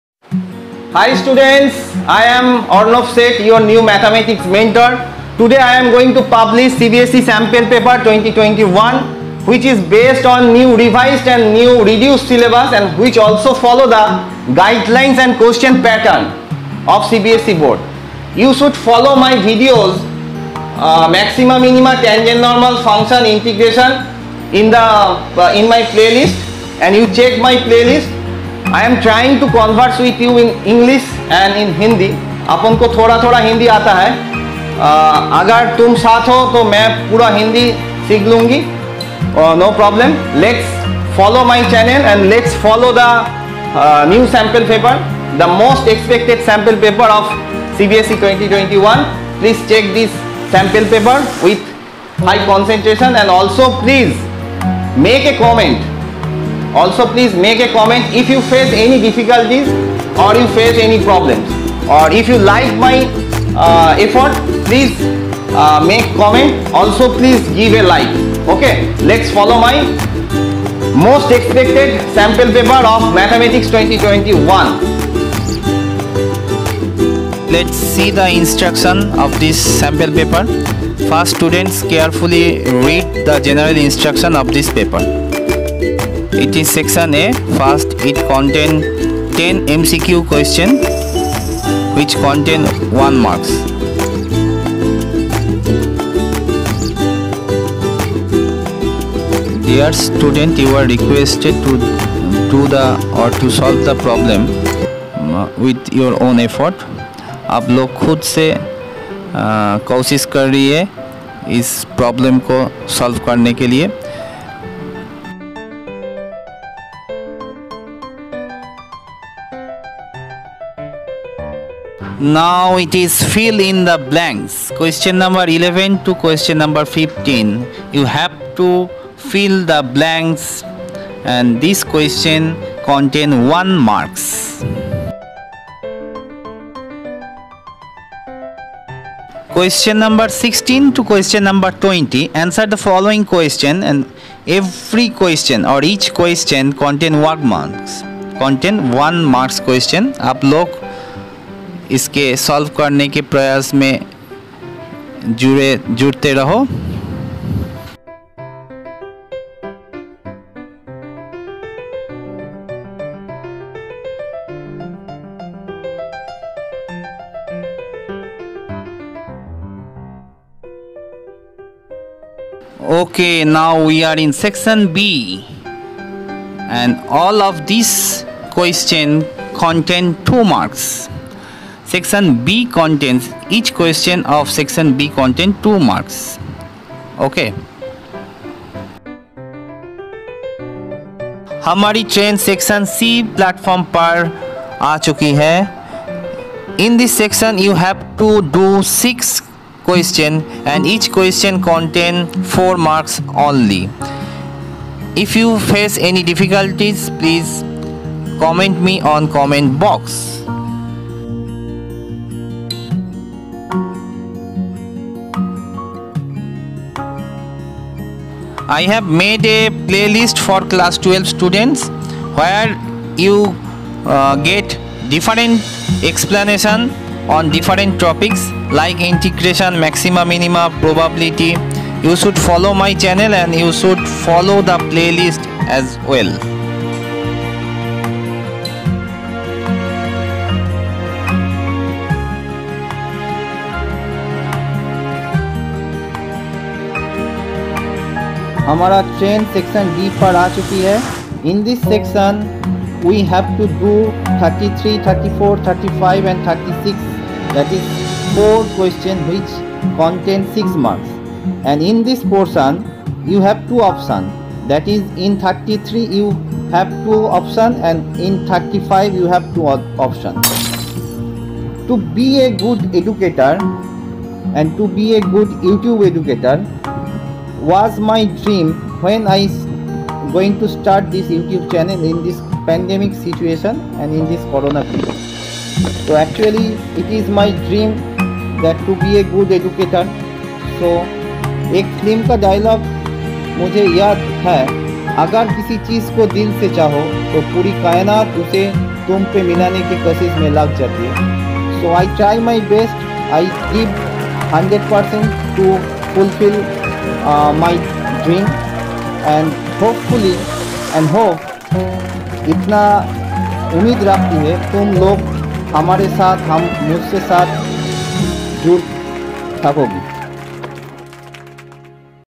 CBSE MATHEMATICS SPECIMEN PAPER 2021/CBSE SAMPLE PAPER OF MATHEMATICS 2021/CBSE MATHS SUGGESTIONS
CBSE BOARD EXAMINATION 2021 MATHEMATICS PAPER . QUESTIONS PATTERN ARE BASED ON CBSE INSTRUCTIONS AND ON NEW
REDUCED AND REVISED SYLLABUS 2021.I ALSO GIVE YOU EXPLANATION OF THESE QUESTIONS PAPER IF YOU FACE ANY PROBLEM.THIS IS A SPECIMEN PAPER OF CBSE2021 FOR CLASS12.CBSEsamplepaper Mathematics CBSE2021 Mathematics sample paper 2021 pdf